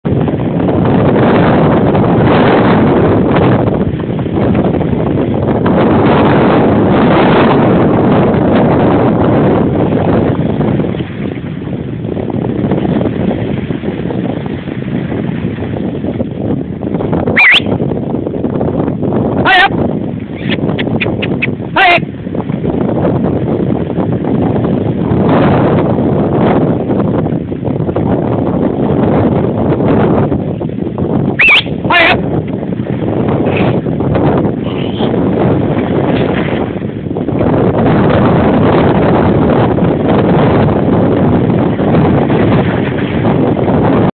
Vídeo020.mp4
Dernier entrainement en neige de la saison 2010 - 2 Avril a Foncine-le-haut, descendant de la Norbière jusqu'a Chez Valentin